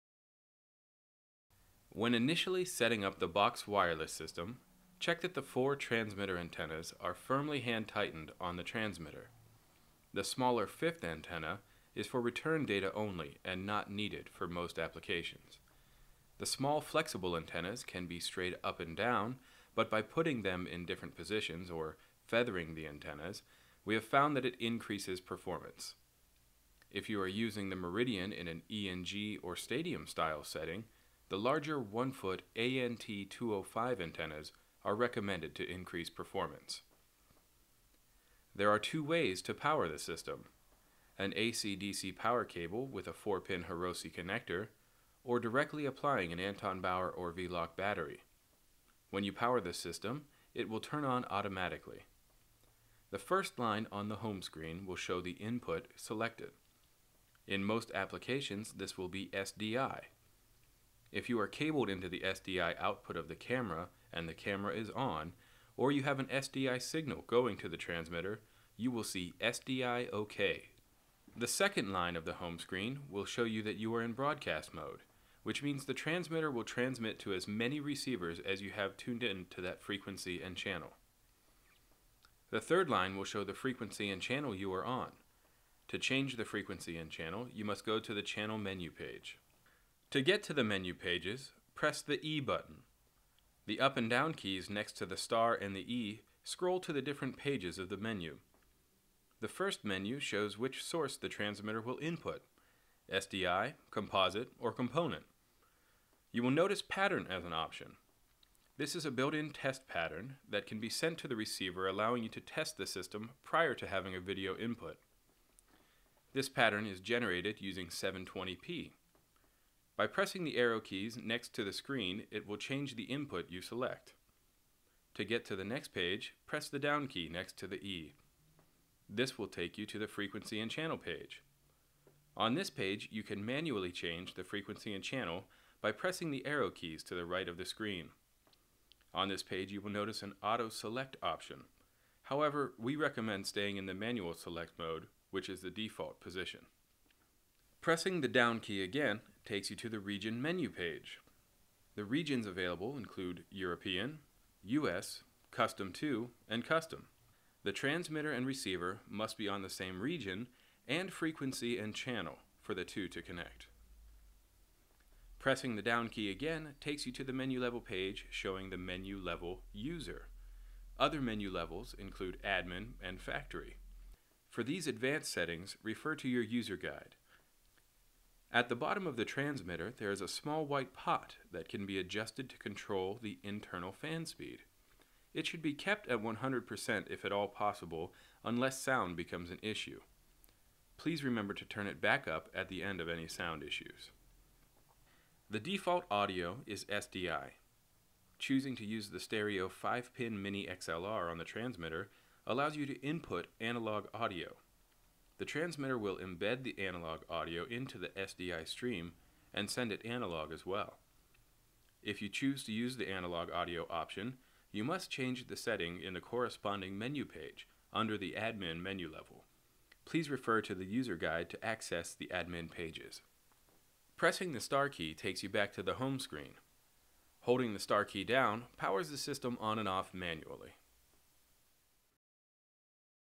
Boxx Meridian Wireless System - TX Tutorial
A quick tutorial about the Boxx Meridian system's transmitter.  The Meridian is Boxx's zero delay, wireless, microwave broadcast system.  Designed for high quality, zero delay HD and SD live transmission, it offers an affordable alternative to traditional COFDM systems.